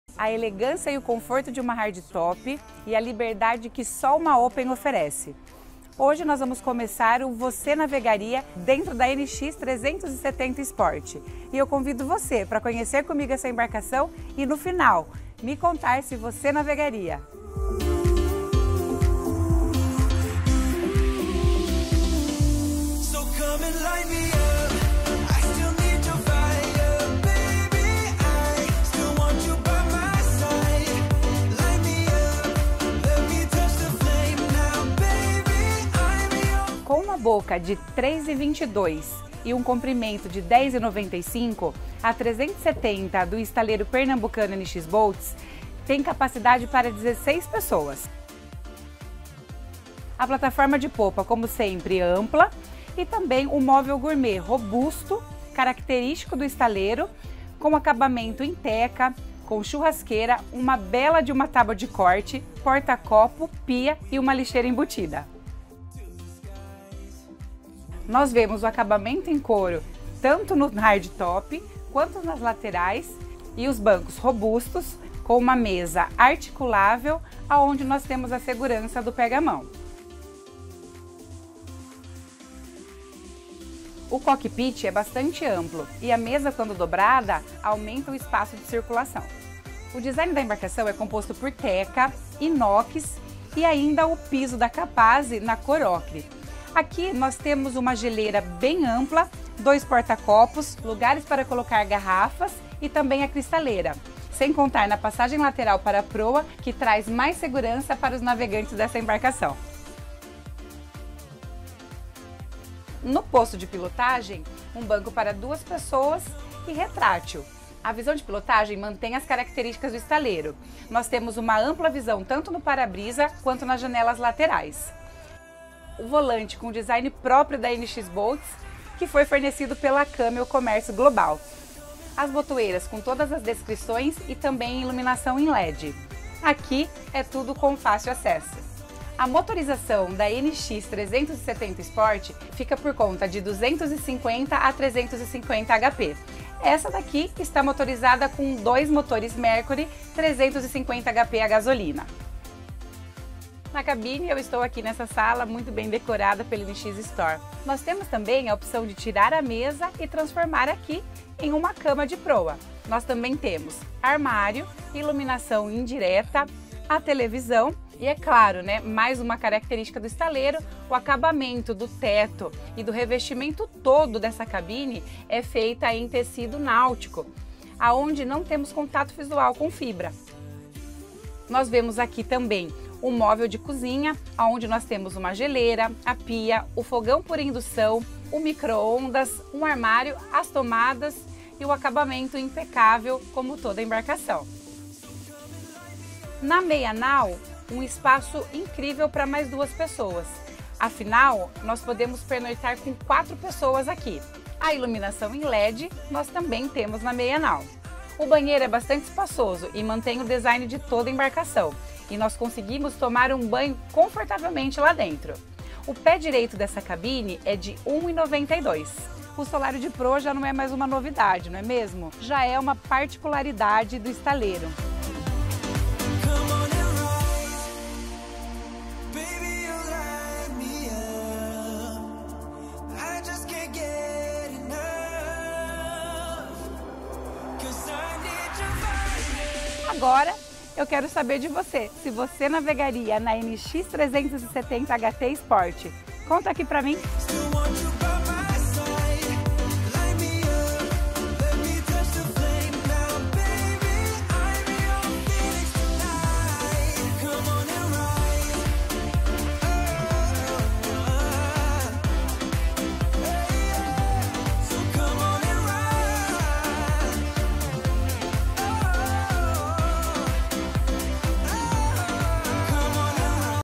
Nx 370 Sport - Você navegaria?
Um barco esportivo e completo, é assim que definimos a Nx 370 Sport. Com área útil totalmente aproveitada, hardtop alto e janelas grandes que proporcionam uma excelente visão panorâmica, esse barco possui capacidade para 16 pessoas, sendo 4 para pernoite.

Comprimento total: 10,95m
Boca: 3,22m
Calado: 0,60m
Altura da Cabine: 1,92m
Altura da Banheiro: 1,85m
Cap. Combustível: 600L
Cap. Agua Doce: 250L
Passageiros: 16 D / 04 N
Motorização: 2x250hp - 2x350hp

Conheça todos os detalhes e deixe nos comentários se VOCÊ NAVEGARIA